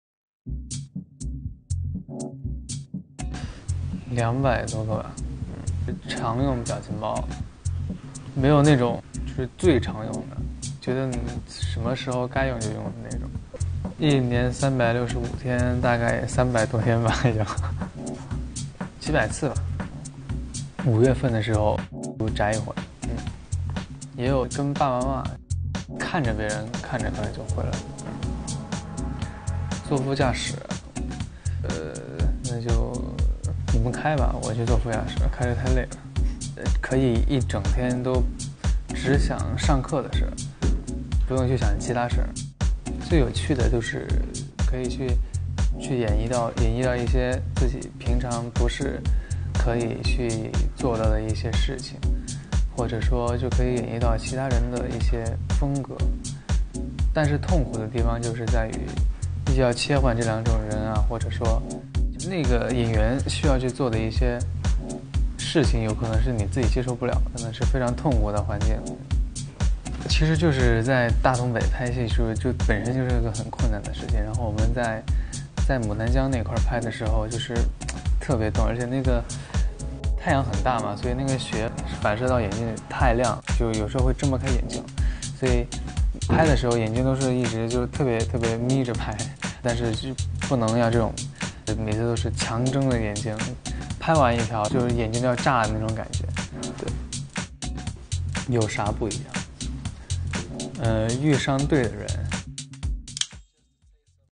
[Vương Tuấn Khải x SuperELLE] Khải nói gì khi Fans muốn ngồi ghế phụ xe anh lái
Vtrans: Yuan
Time: Lam
Typeset + Encode: Táo